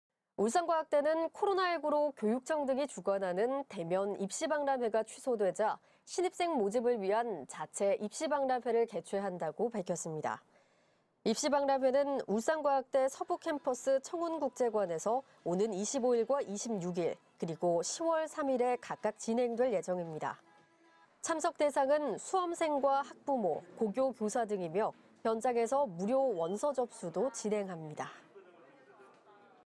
울산과학대, 2022학년도 신입생 모집 입시박람회 개최 20210921
울산과학대, 2022학년도 신입생 모집 입시박람회 개최

울산과학대는 코로나19로
교육청 등이 주관하는 대면
입시박람회가 취소되자, 신입생
모집을 위한 자체 입시박람회를
개최한다고 밝혔습니다.

 입시박람회는 울산과학대
서부캠퍼스 청운국제관에서
오는 25일과 26일,
그리고 10월 3일에
각각 진행될 예정입니다.

 참석 대상은
수험생과 학부모 고교 교사
등이며 현장에서 무료
원서접수도 진행합니다.@@